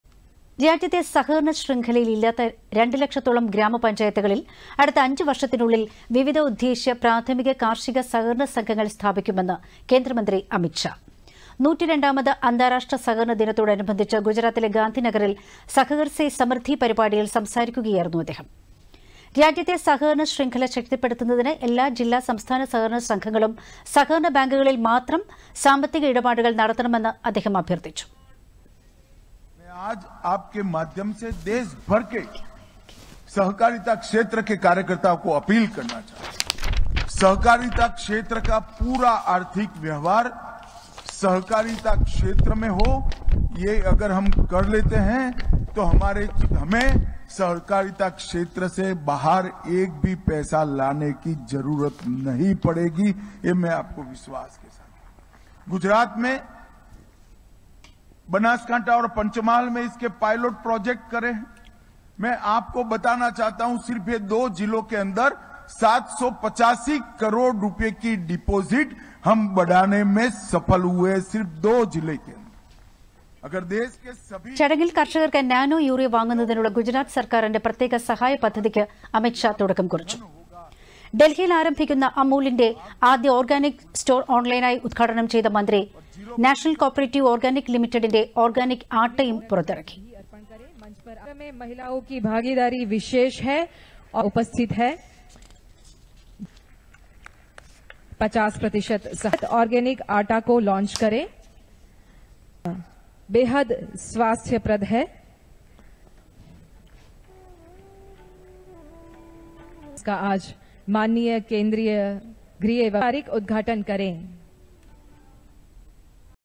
സഹകര്‍ സേ സമൃദ്ധി || 102-ാമത്‌ അന്താരാഷ്ട്ര സഹകരണ ദിനം || അമിത് ഷാ
രാജ്യത്തെ സഹകരണ ശൃംഖലയില്ലാത്ത രണ്ട് ലക്ഷത്തോളം ഗ്രാമ പഞ്ചായത്തുകളിൽ അടുത്ത അഞ്ച് വർഷത്തിനുള്ളിൽ വിവിധോദ്ദേശ്യ പ്രാഥമിക കാർഷിക സഹകരണ സംഘങ്ങൾ സ്ഥാപിക്കുമെന്ന് കേന്ദ്രമന്ത്രി അമിത് ഷാ.

102-ാമത്‌ അന്താരാഷ്ട്ര സഹകരണ ദിനത്തോടനുബന്ധിച്ച്‌ ഗുജറാത്തിലെ ഗാന്ധിനഗറില്‍ സഹകര്‍ സേ സമൃദ്ധി പരിപാടിയില്‍ സംസാരിക്കുകയായിരുന്നു അദ്ദേഹം.

രാജ്യത്തെ സഹകരണ ശൃംഖല ശക്തിപ്പെടുത്തുന്നതിന് എല്ലാ ജില്ലാ സംസ്ഥാന സഹകരണ സംഘങ്ങളും സഹകരണ ബാങ്കുകളിൽ മാത്രം സാമ്പത്തിക ഇടപാടുകൾ നടത്തണമെന്ന് അദ്ദേഹം അഭ്യർത്ഥിച്ചു.

ചടങ്ങിൽ കർഷകർക്ക് നാനോ യൂറിയ വാങ്ങുന്നതിനുള്ള ഗുജറാത്ത് സർക്കാരിന്റെ പ്രത്യേക സഹായ പദ്ധതിക്ക് അമിത്ഷാ തുടക്കം കുറിച്ചു.

ഡൽഹിയിൽ ആരംഭിക്കുന്ന അമൂലിന്റെ ആദ്യ ഓർഗാനിക് സ്റ്റോർ ഓൺലൈനായി ഉദ്ഘാടനം ചെയ്ത മന്ത്രി ,നാഷണൽ കോപ്പറേറ്റിവ് ഓർഗാനിക് ലിമിറ്റഡിന്റെ ഓർഗാനിക് ആട്ടയും പുറത്തിറക്കി.